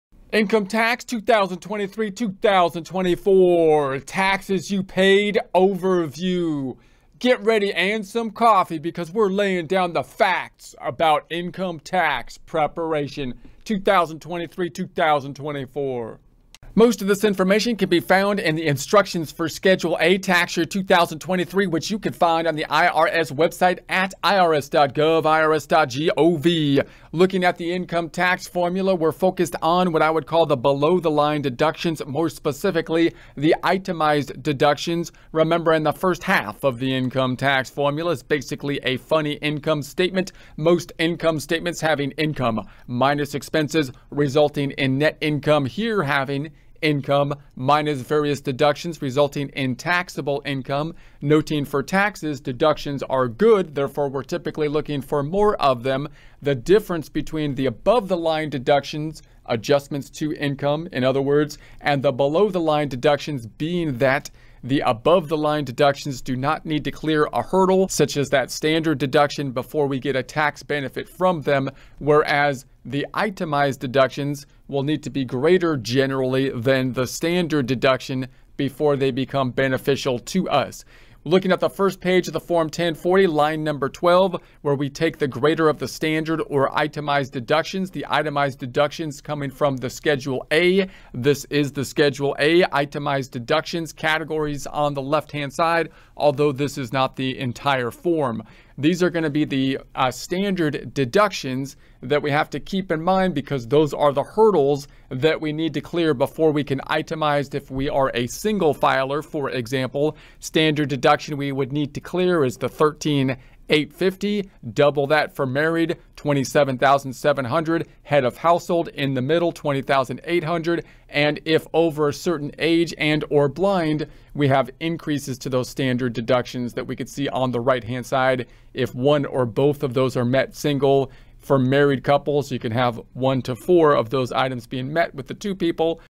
Introduction to income tax preparation 2023-2024
"Get ahead on your taxes! Dive into our concise guide to income tax preparation for the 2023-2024 fiscal year. Learn essential tips, changes, and strategies to streamline your filing process and maximize your returns. Stay informed and stay ahead with our comprehensive overview!"